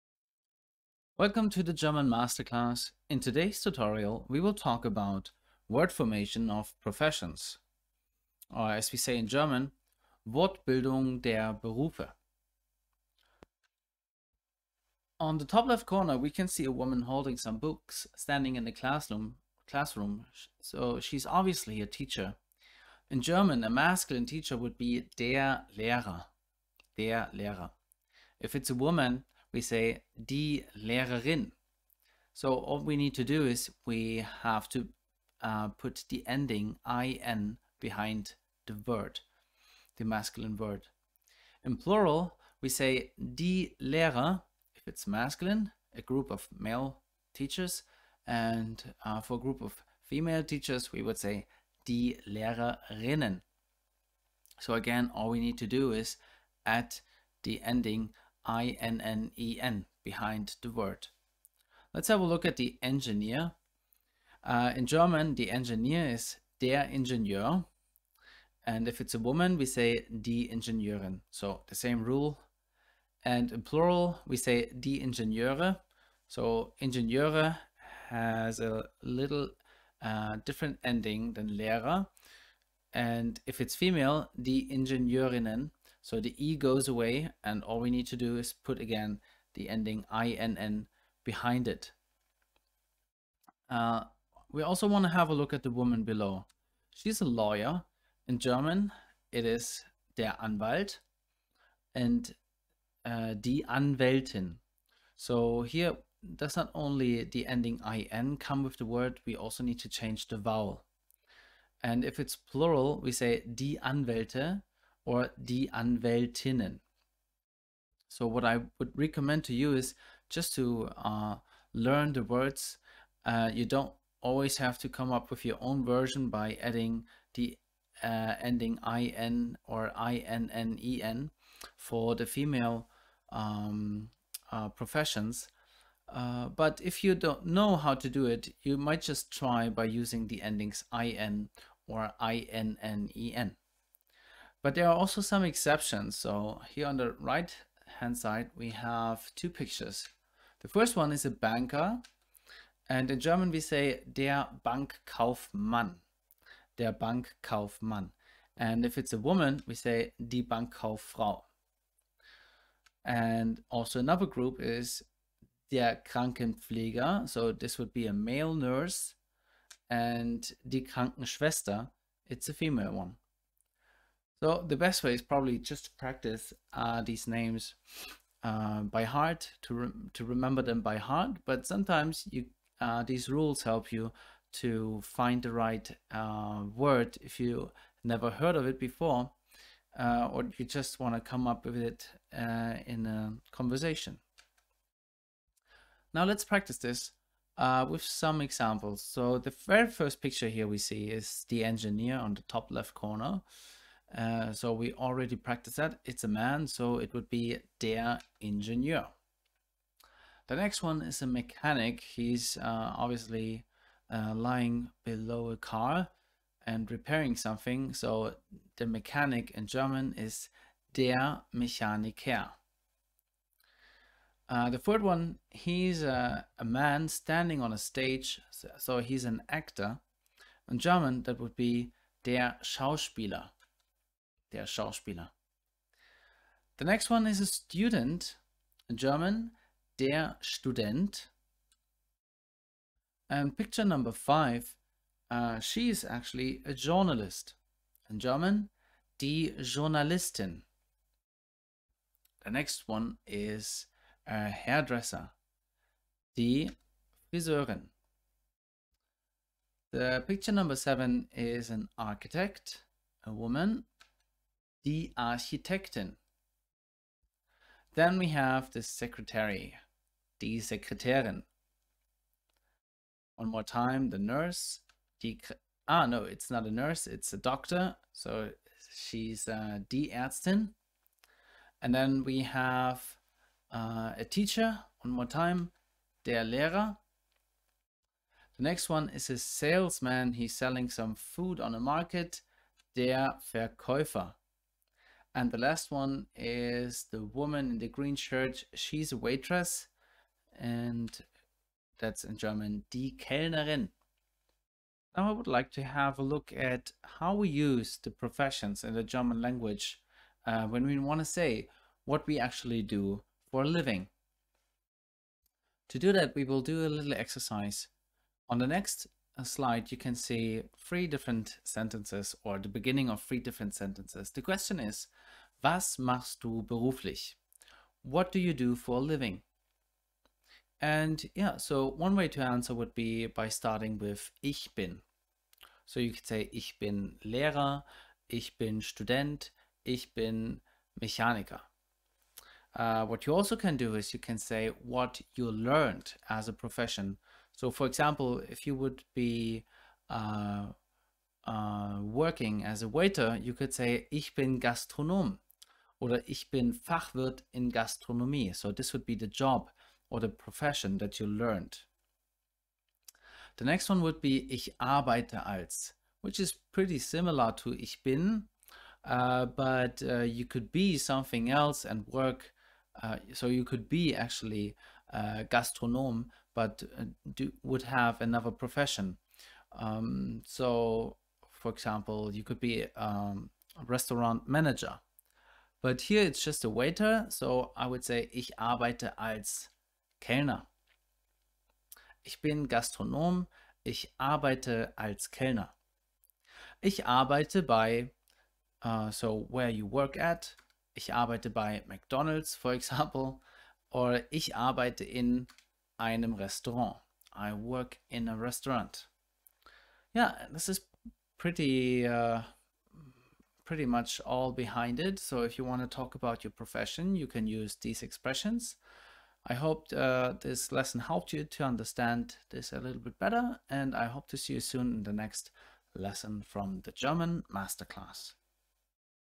Word formation professions | Wortbildung: männliche und weibliche Berufe | German Lesson Level 1
In this tutorial from The German Masterclass, you will learn how to form male and female professions in german and how to say what your profession is.